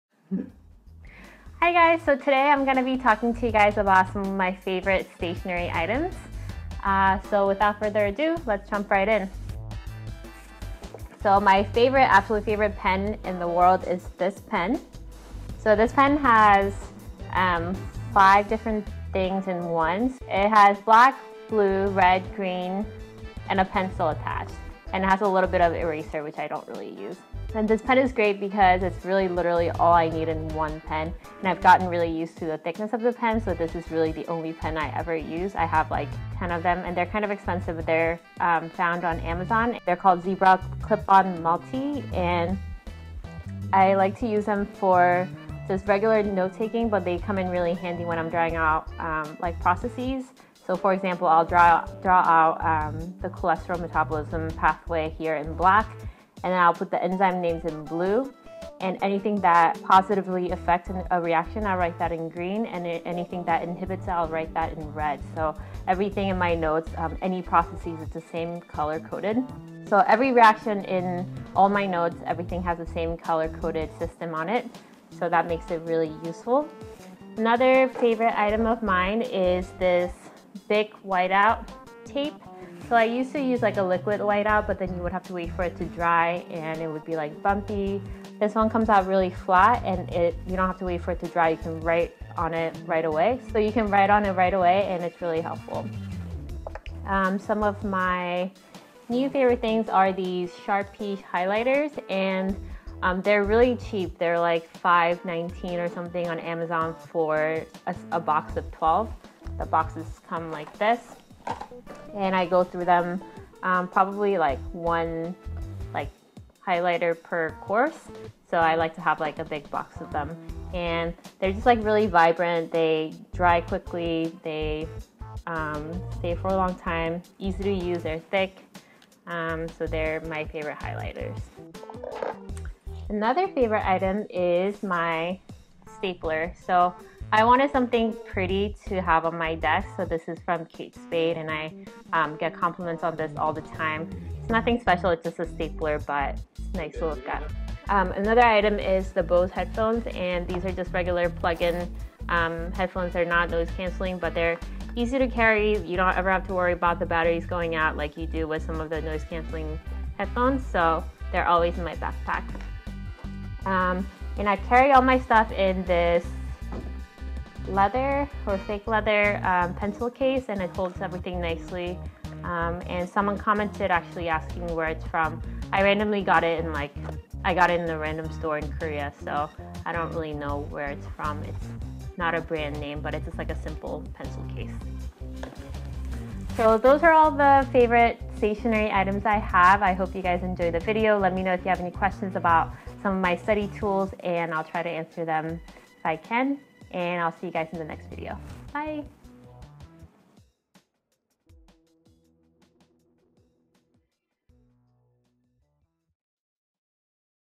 My Favorite Note Taking Tools and Stationery For Medical School
Taking notes in medical school or in college can sometimes be a chore. With the right stationery I am able to take notes in a way that helps me understand my medical school materials better. I have listed the tools I use below, I hope you guys find them just as useful as I found them.

Here are the links:
1. Zebra Clip on Multi Pen:

2. Kate Spade New York Acrylic/Gold Stapler:

3. Sharpie Accent Tank-Style Highlighters, Fluorescent Yellow, 12 Pack:

4. BIC Wite-Out Mini Twist Correction Tape, White

6. Bose® MIE2i Mobile Headset:

Mailing Address:
Jamie L.